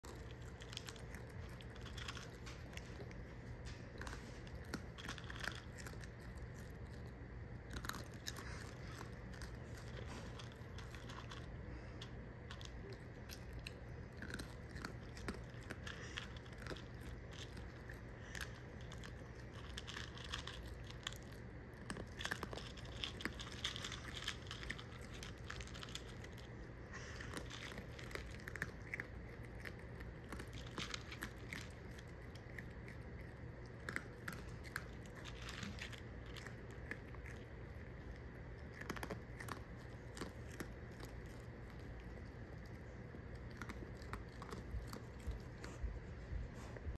Little Raccoon Enjoys a Bedtime Snack || ViralHog
Occurred on August 6, 2023 / Maumelle, Arkansas, USA

Lillie the raccoon enjoying a bedtime snack.